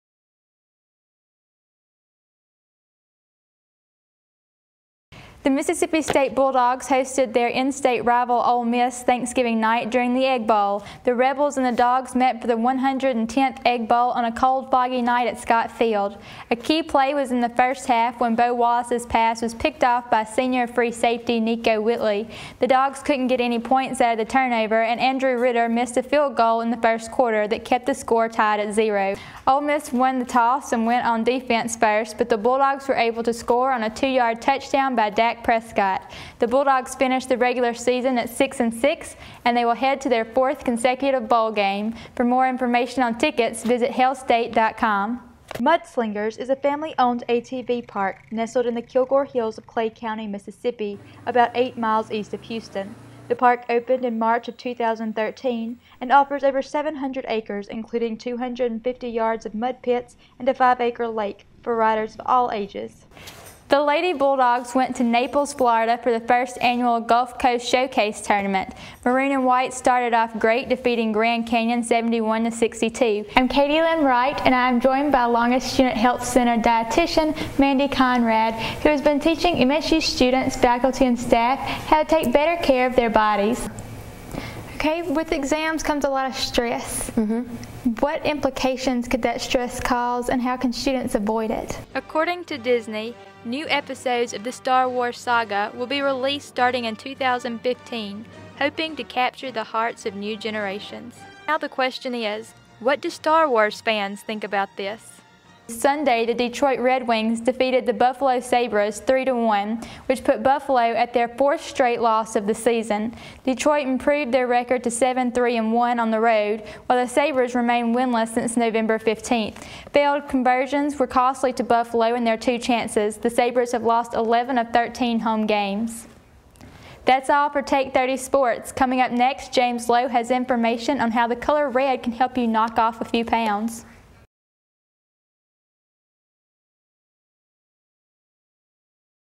Katylynn Wright News and Sports Anchor Reporter Package Reel
This is a short resume reel featuring some of my sports anchoring, news reporting/news packages with voice overs, and interviews. Everything in this video was completed between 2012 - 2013 while I was attending Mississippi State University.